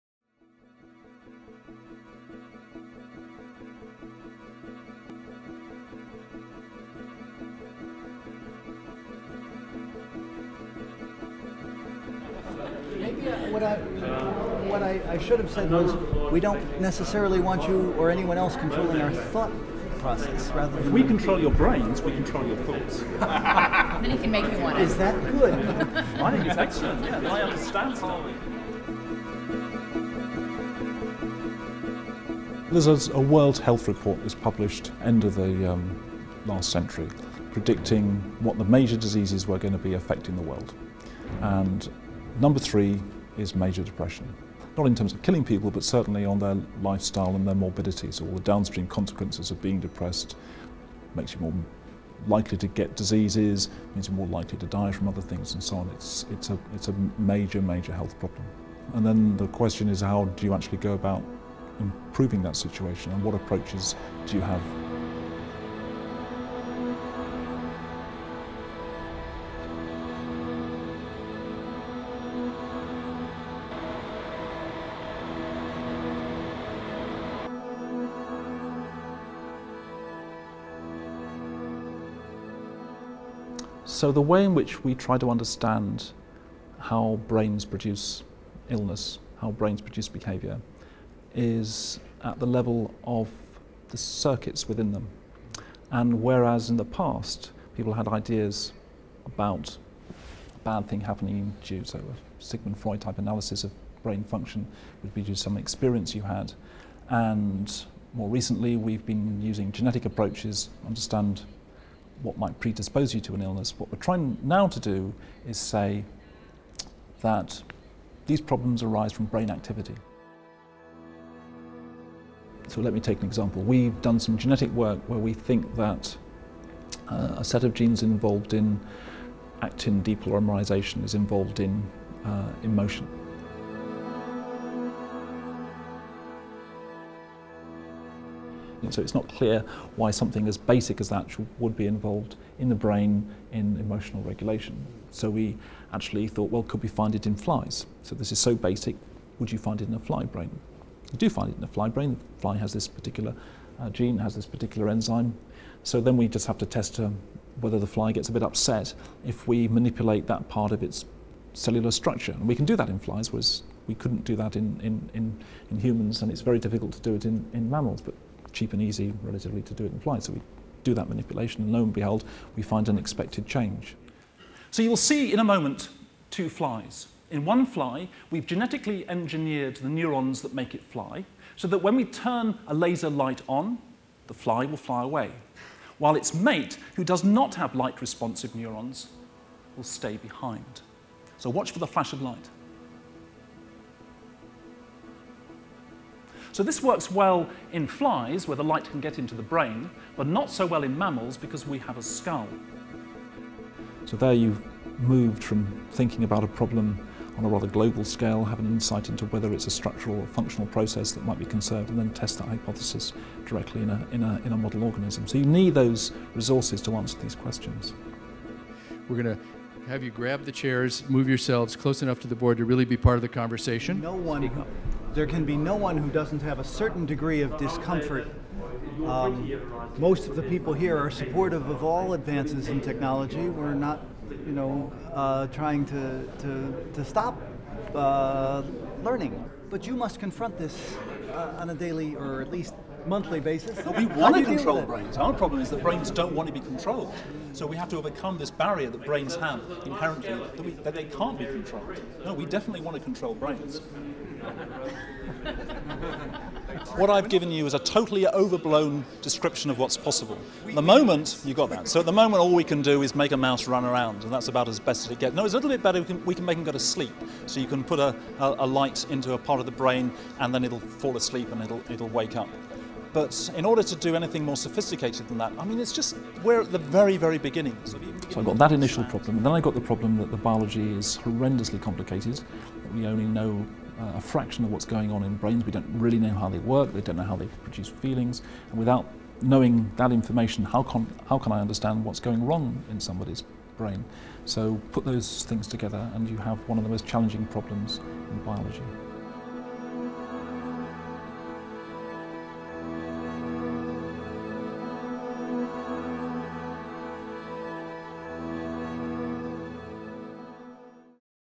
IdeasLabs 2011 - Curing Unhappiness - Jonathan Flint
Jonathan Flint is discovering how to change brain behaviour to prevent depression and other mental illness. The World Health Organization has identified depression as the potential cause of a major health crisis in terms of impact on population health, quality of life and susceptibility to other diseases. Understanding the biological basis of common disorders such as depression and anxiety is a starting point for developing effective therapies. Jonathan Flint is Co-Director for the Programme for Mind and the Machine at Oxford University.